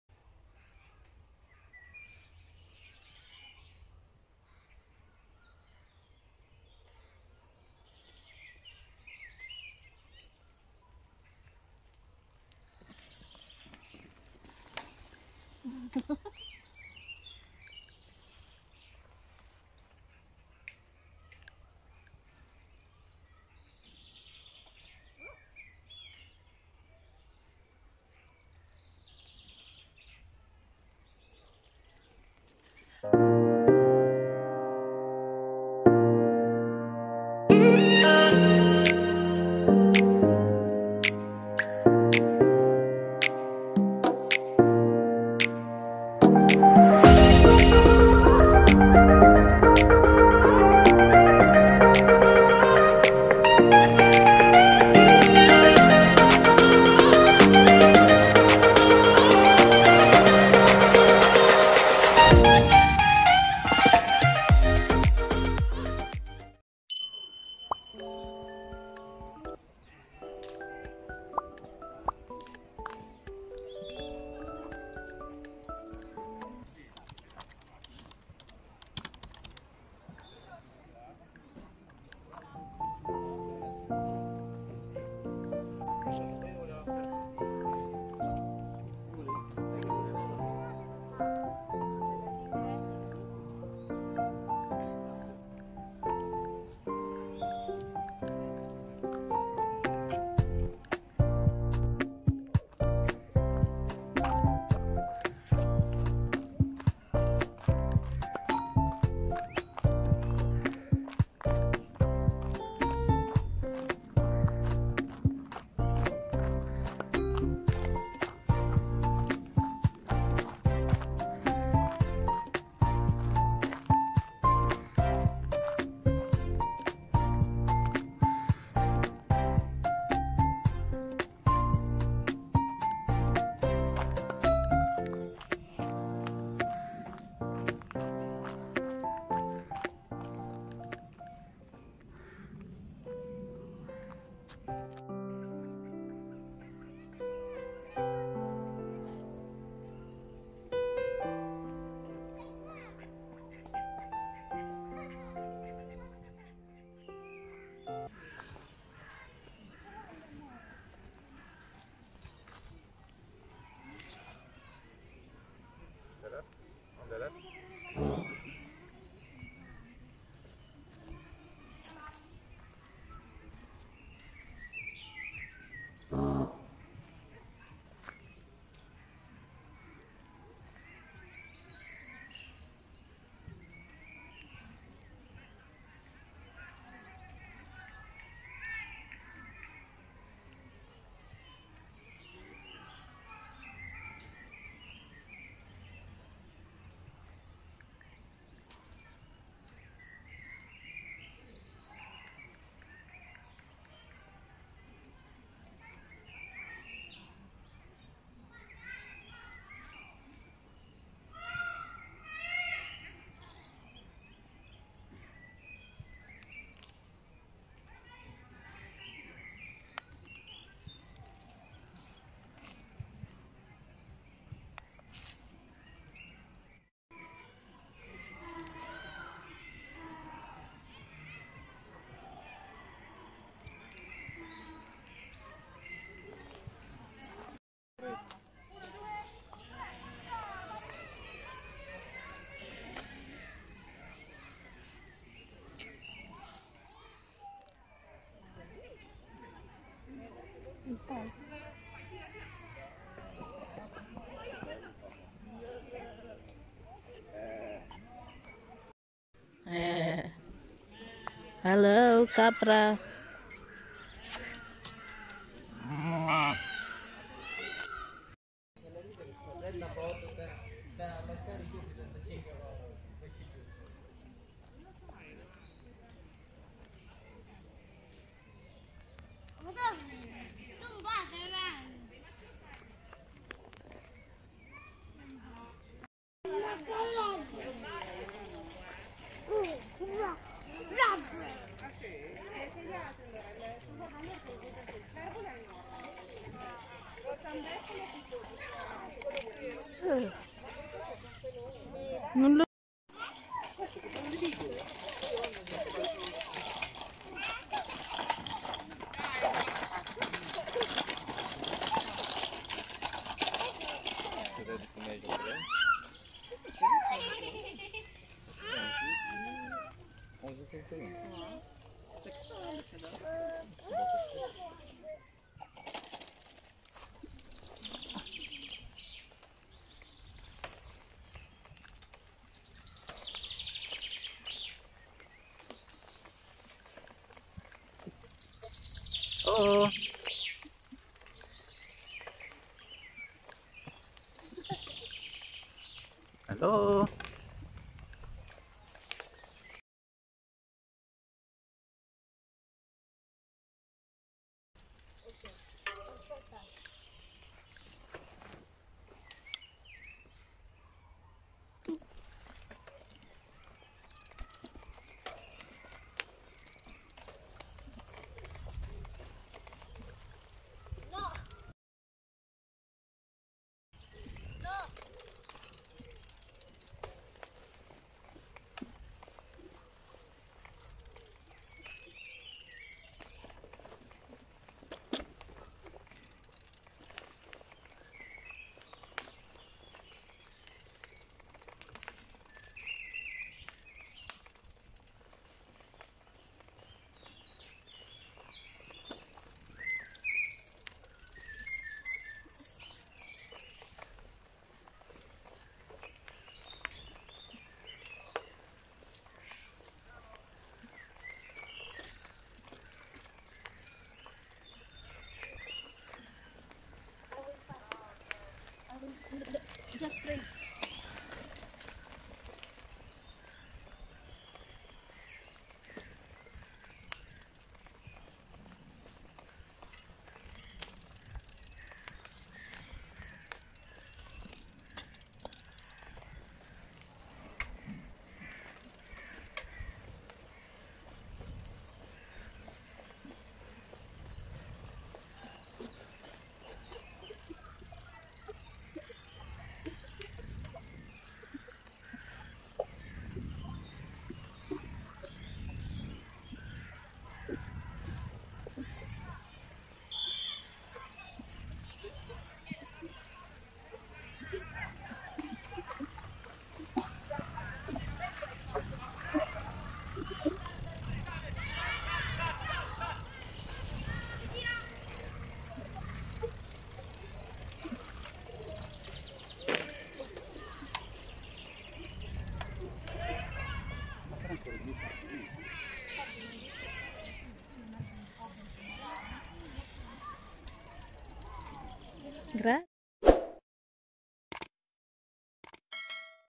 Parco Arcadia - Bareggio, Milan Italy
We were cycling from Milan to Bareggio for more than 2 hours ( I was slow- my first time cycling long sistance). We ended up in Parco Arcadia, saw some animals and relaxed a bit then went back to Milan. This time it took us only an hour.